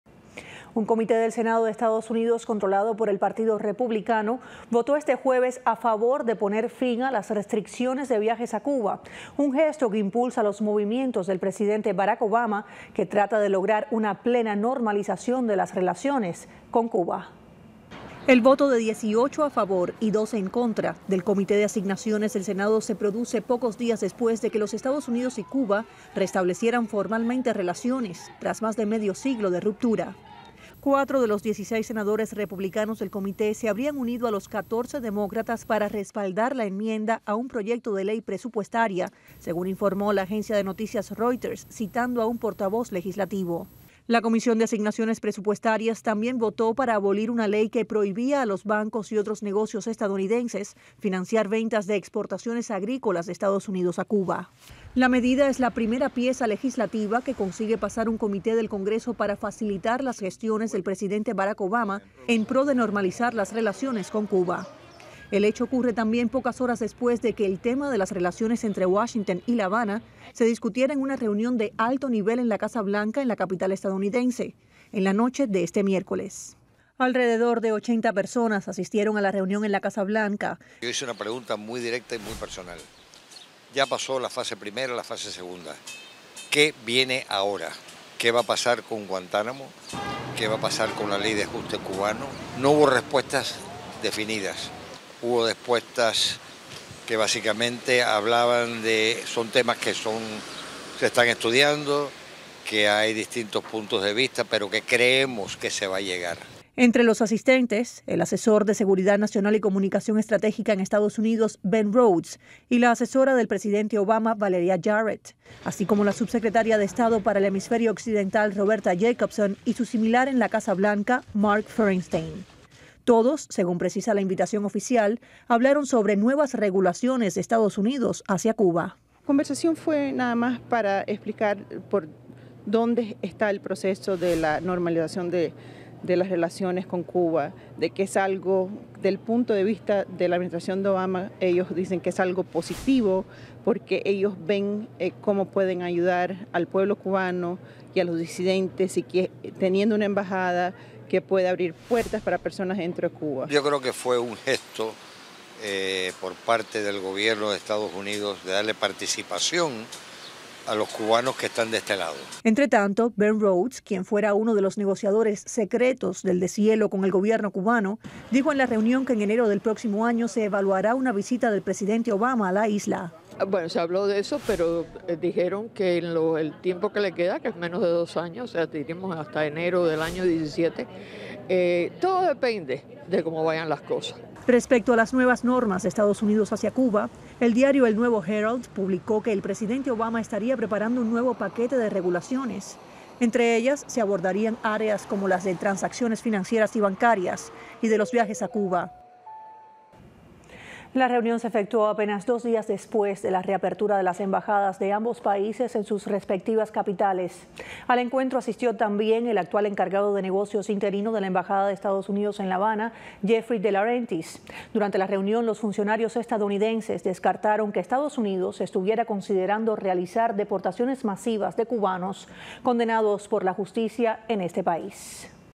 Comité del Senado de EU vota a favor de poner fin a restricciones de viajes a Cuba
La votación impulsa la gestión de la Casa Blanca a favor de la normalización de las relaciones con Cuba. Karen Caballero tiene los detalles